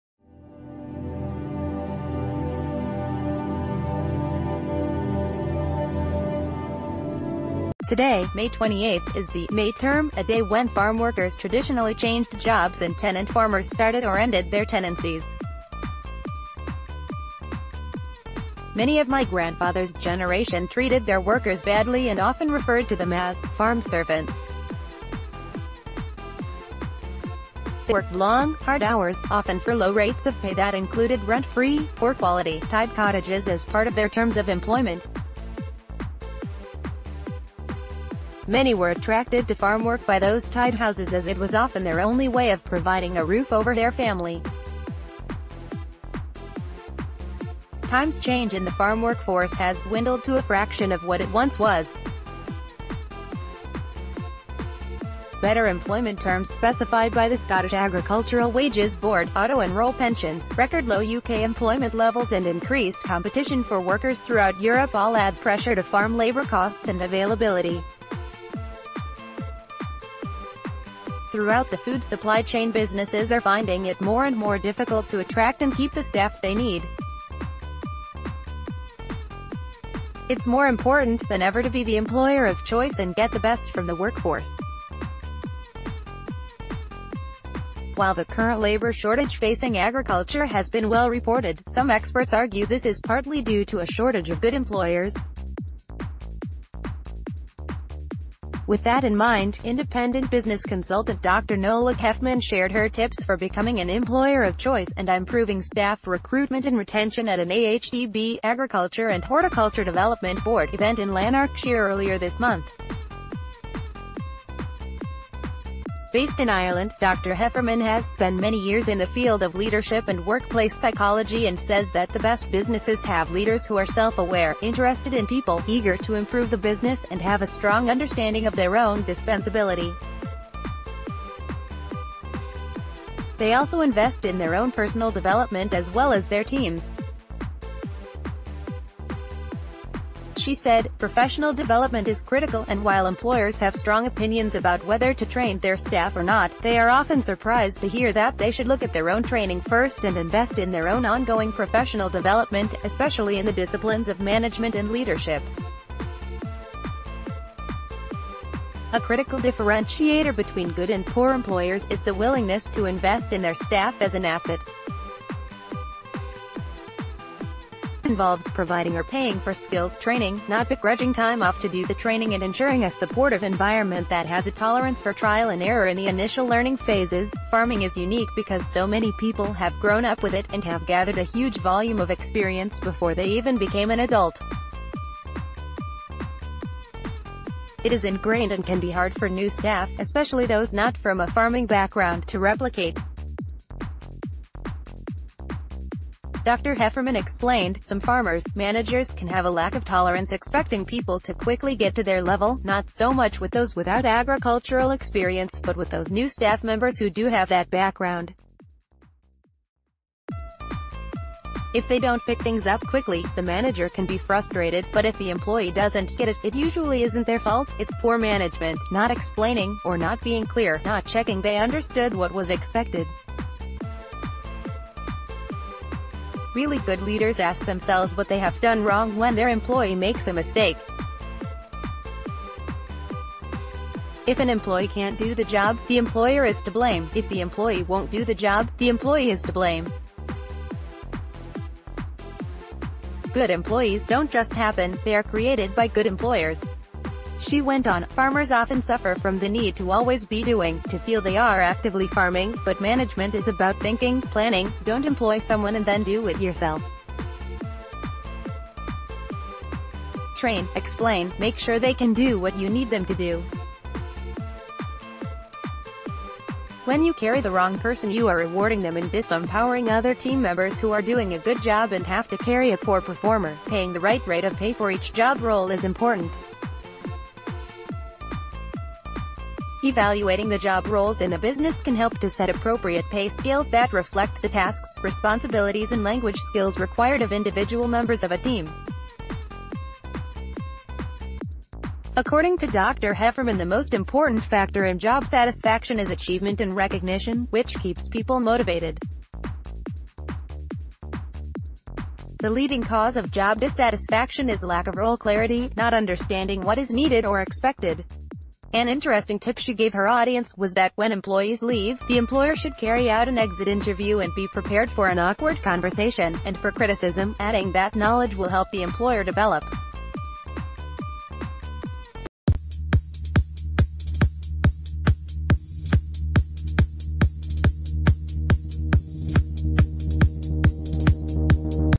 Breaking News | Rog Wood: 'Paying the right rate of pay for each job role is important'
Today, May 28th, is the "May Term", a day when farm workers traditionally changed jobs and tenant farmers started or ended their tenancies. Many of my grandfather's generation treated their workers badly and often referred to them as "farm servants". They worked long, hard hours, often for low rates of pay that included rent-free, poor-quality, tied cottages as part of their terms of employment. Many were attracted to farm work by those tied houses as it was often their only way of providing a r...